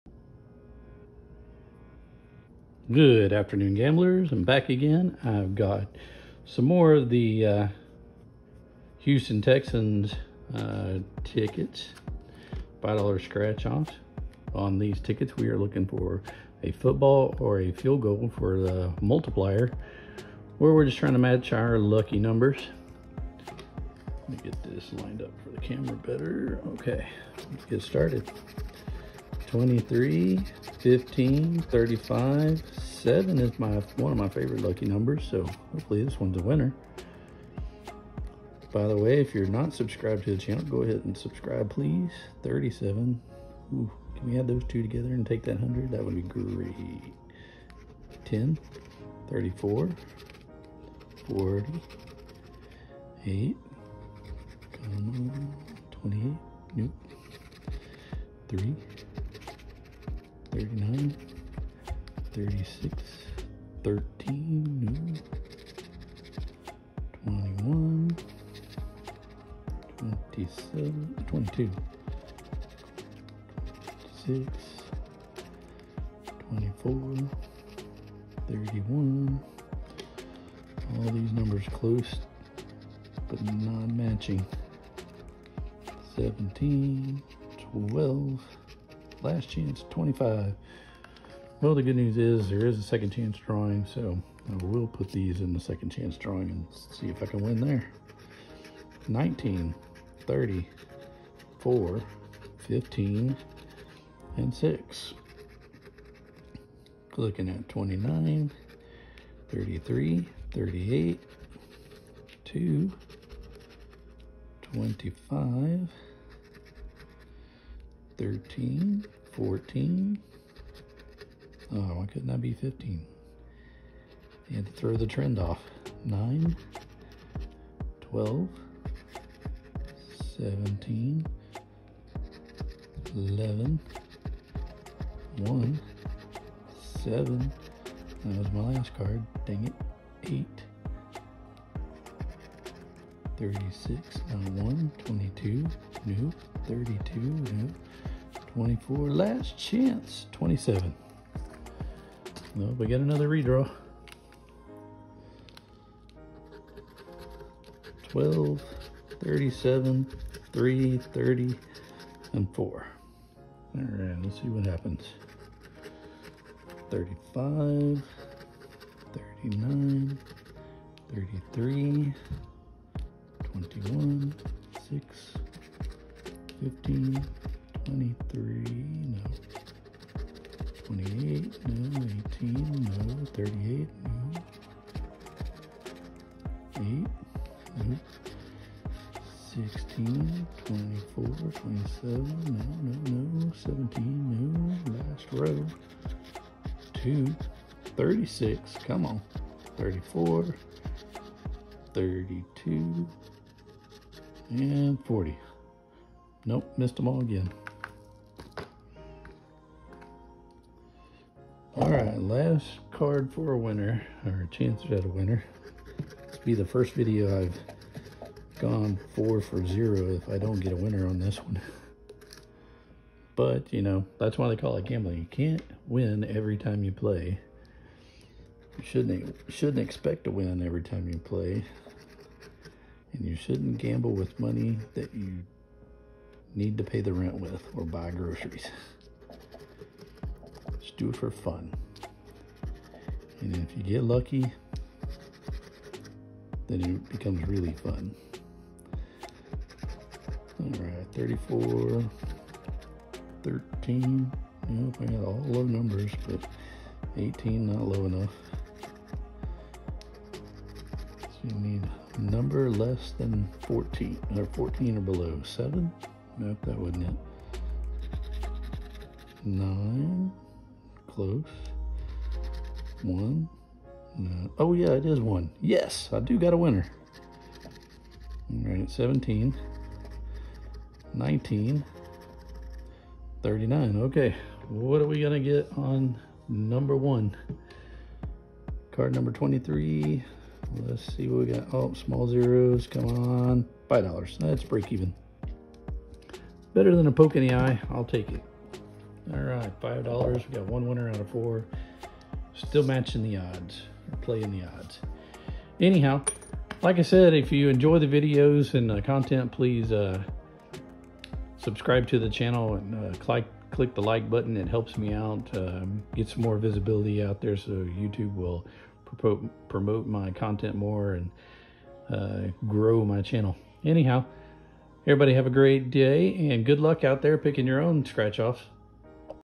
Texas Gambler Houston Texans Scratch Off Tickets
Texas Gambler Vlog 22 - Houston Texans tickets... Hope I get a second chance prize!
Texas Lotto Scratch off tickets, scratching for your entertainment.  Please gamble responsibility and don't use your rent money!